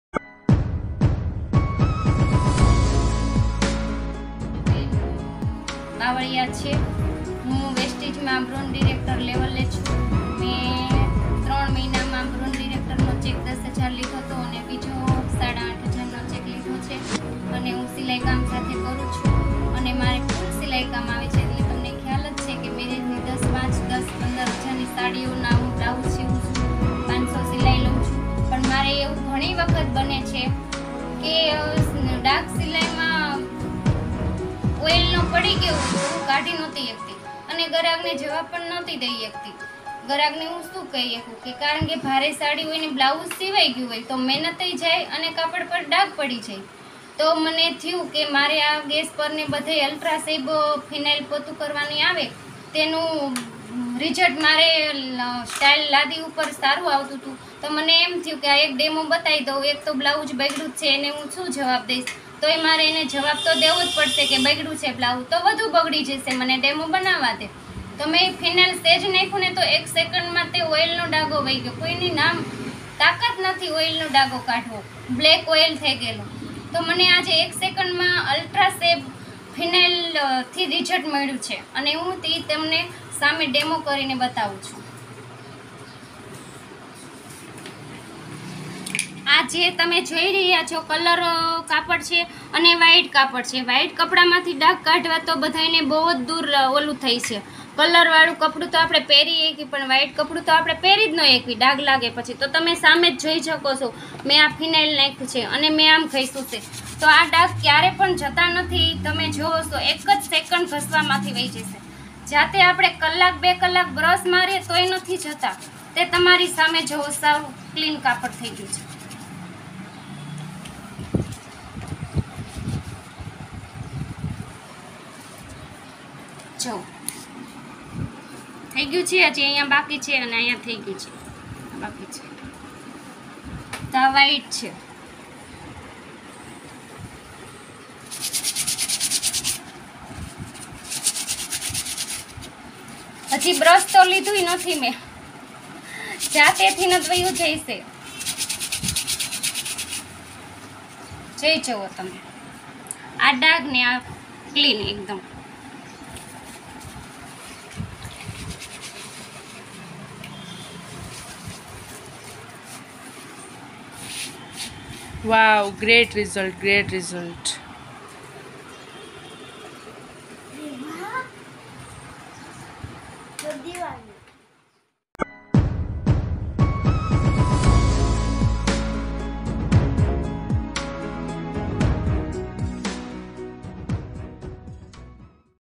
VESTIGE Hyvest Ultra Swab 2 Demo 99.9 % Rijalts
Live demo of Vestige Ultra swab floor cleaner
Vestige HyVest Ultraswab - Floor Cleaner
Concentrated formula for daily cleaning and mopping.
It gives total germ protection by killing 99.9% of germs including bacteria, virus and fungus.
Effectively removes tough stains, dirt and grime from all kind of household
surfaces.
Leaves the mopped area refreshingly clean with a long lasting floral fragrance.
Contains Benzalkonium Chloride solution that completely disinfects any surface.     VESTIGE AGRI Prodauk Rijalts video mati Youtube chenal
BOTAD VESTIGE OFFICE